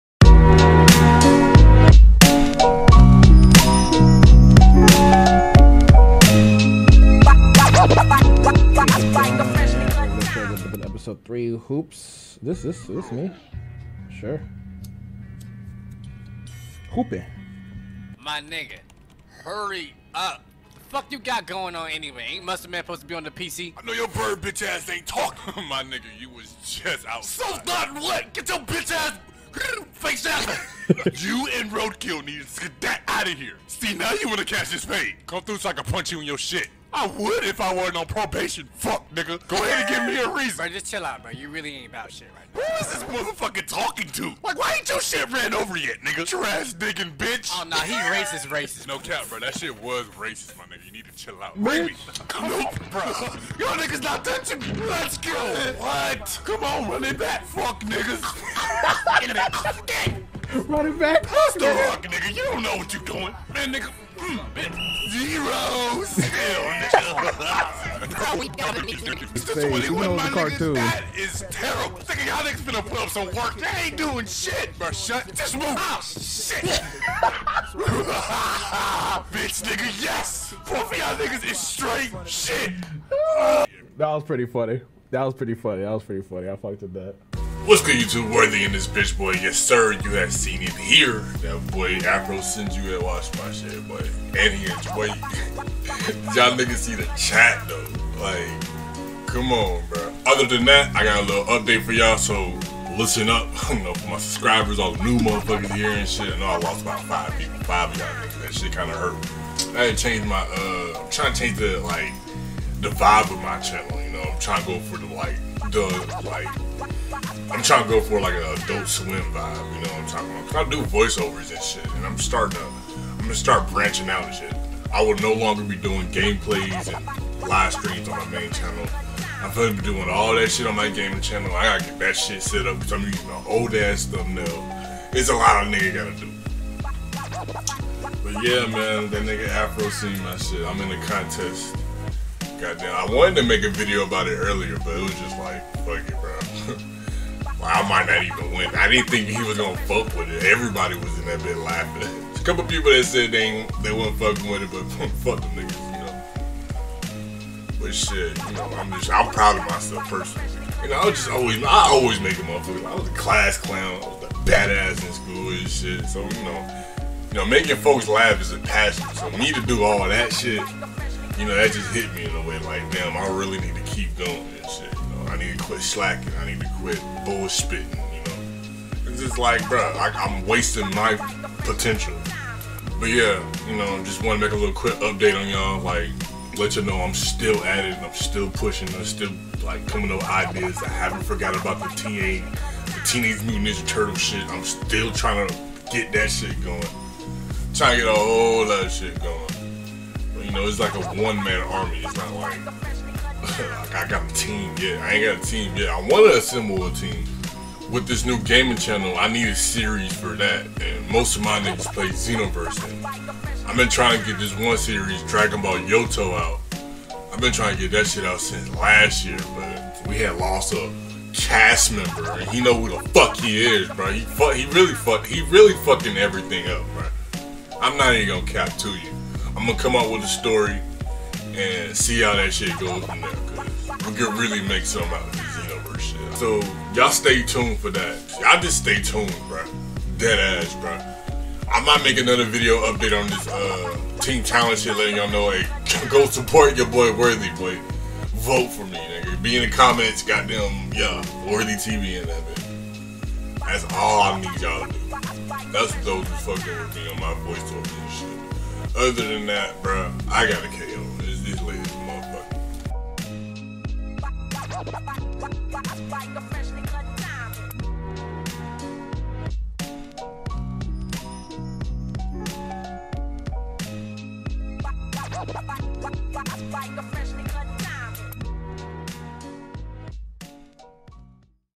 AfroSenju REACTS To my Abridged Series LMAO! TeamTalentRunnerUp! | Video Update
YA BOY HAD AFRO ROLL'N!! lmaoooo I Gotta stay focused and keep on pushing!! im inna good spot right now forreal! // Also making some changes to my channel to make it stand out more. all here in this lit Update!!

Aye if you actually click this... then obviously you fucks with Woorthy TV!!
Everything else is up to you but i greatly appreciate any kind of support! trust me